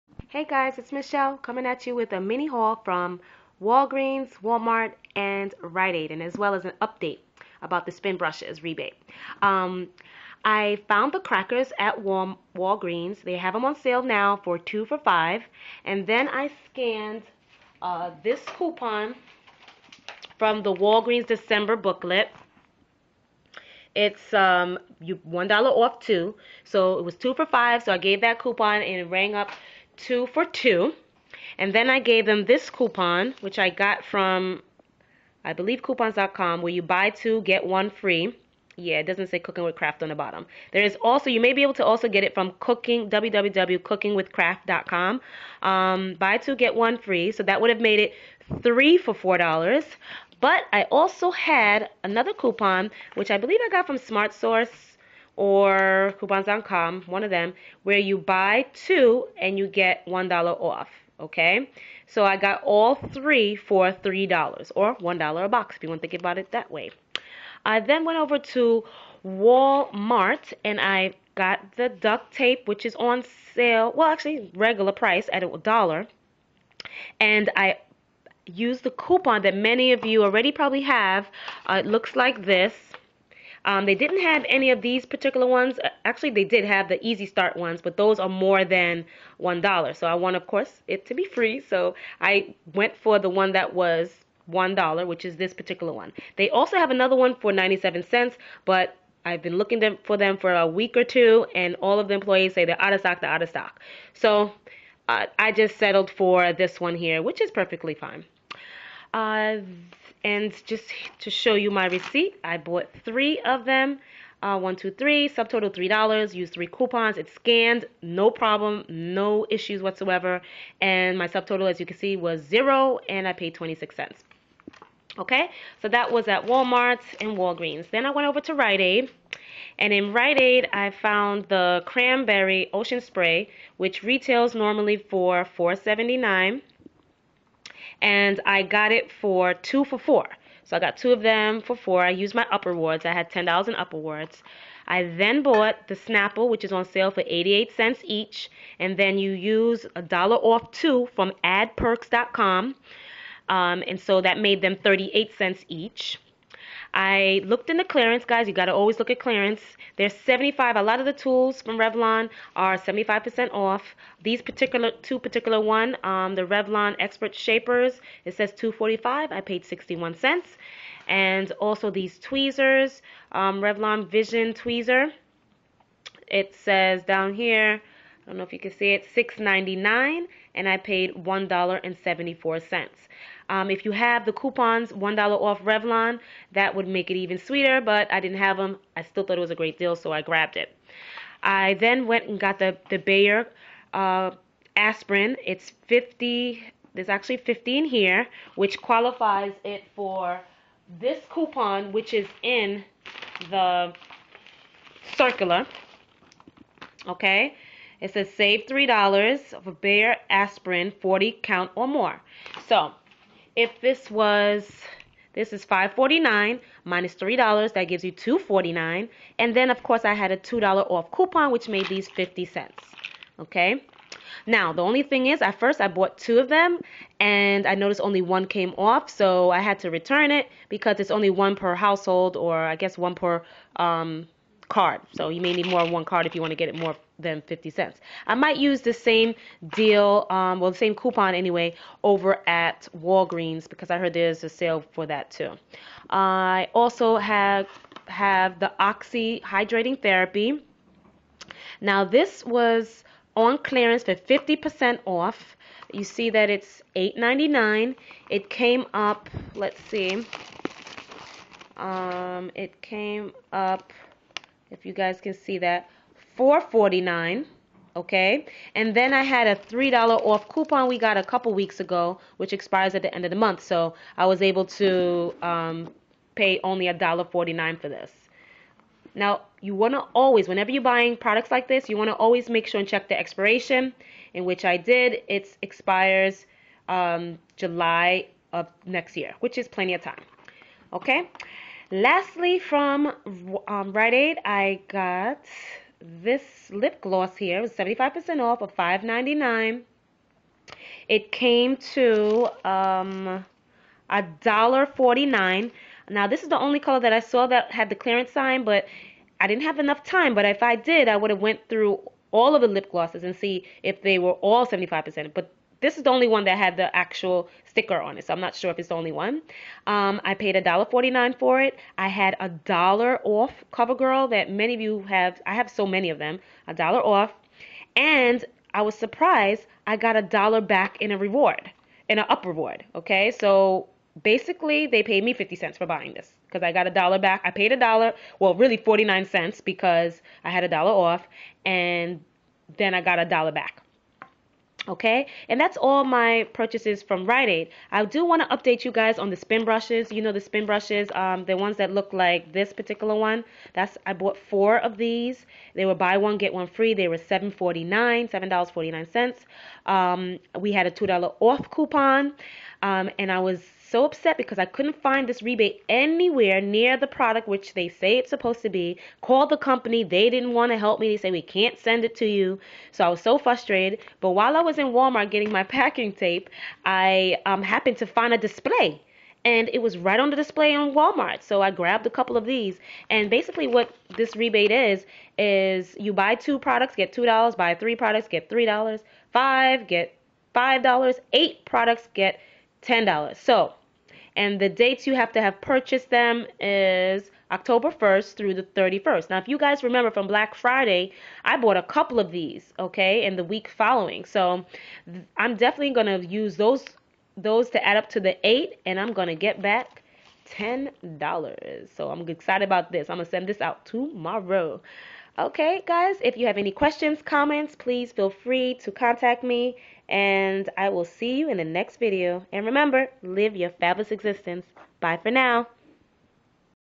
Walmart, Walgreens, and RiteAid Deals 12/21
Hey guys check out out my midweek mini haul.